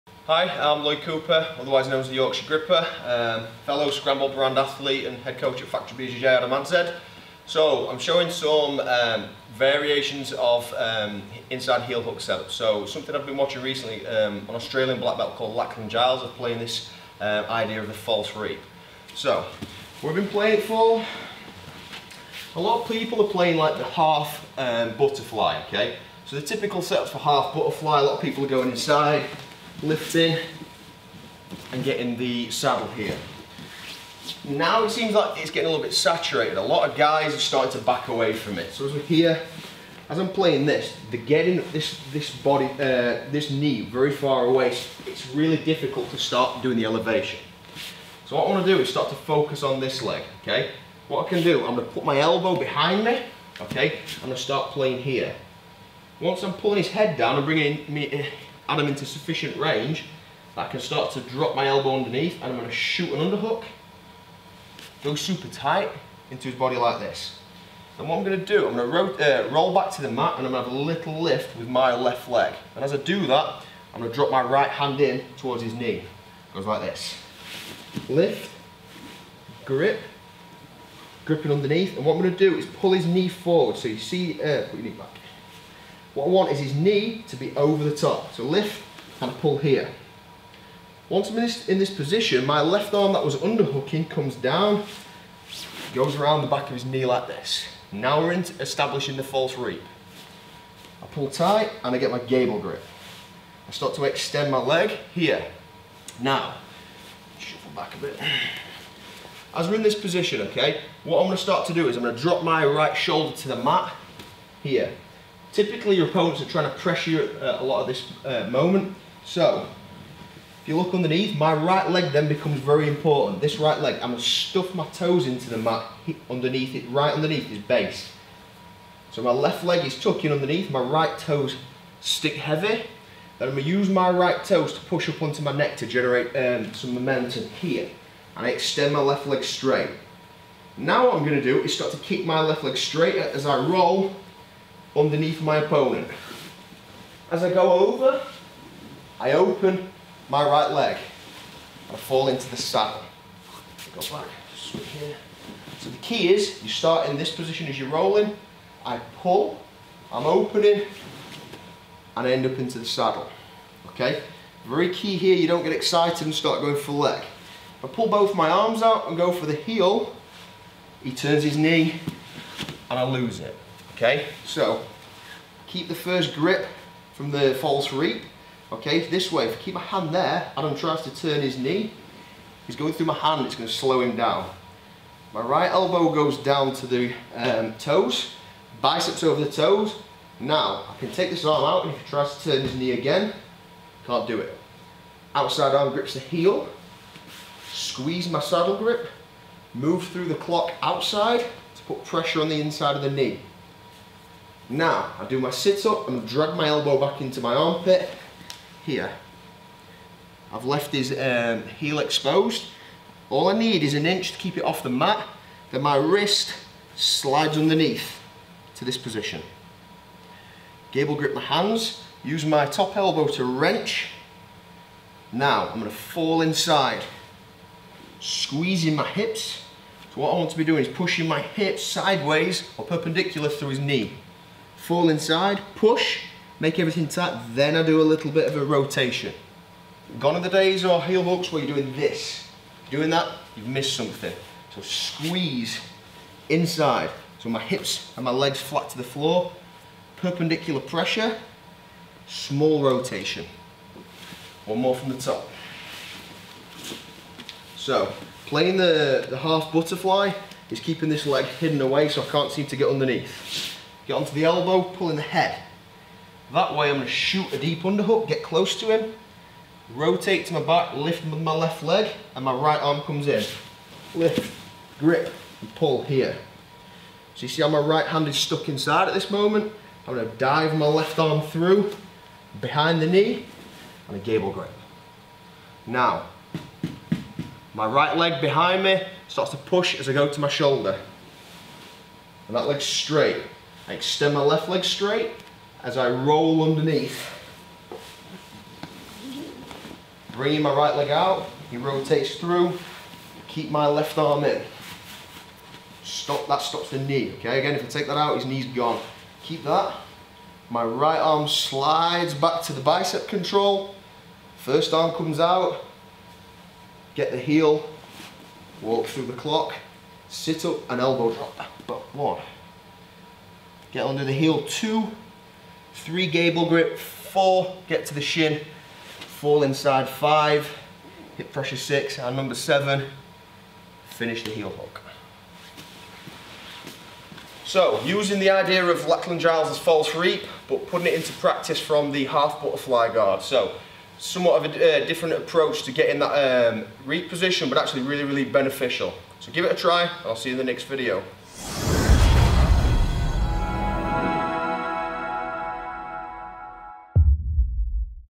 No Gi Jiu Jitsu Submissions - Inside Heel Hook from Half Butterfly Tutorial
No Gi Jiu Jitsu Submission, inside heel hook from the half butterfly with some extra funky coaching details for your viewing pleasure!
for more amazing videos!

Lloyd Cooper, Top UK BJJ Black Belt, Scramble sponsored athlete, highly regarded Gi/No Gi grappler and head coach of Combat Base Goole is the featured instructor here filmed by the Warrior Collective